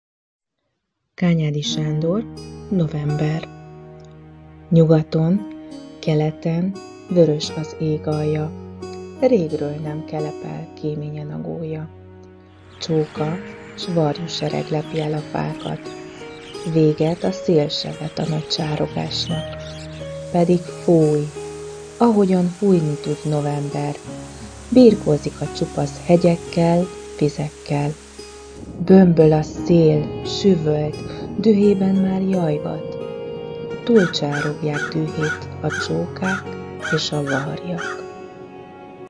Kányádi Sándor - November
Nyugaton, keleten
vörös az ég alja.
Régről nem kelepel
kéményen a gólya.

Csóka- s varjúsereg
lepi el a fákat,
véget a szél se vet
a nagy csárogásnak.

Pedig fúj, ahogyan
fújni tud november,
birkózik a csupasz
hegyekkel, vizekkel.

Bömböl a szél, süvölt,
dühében már jajgat:
túlcsárogják dühét
a csókák és a varjak.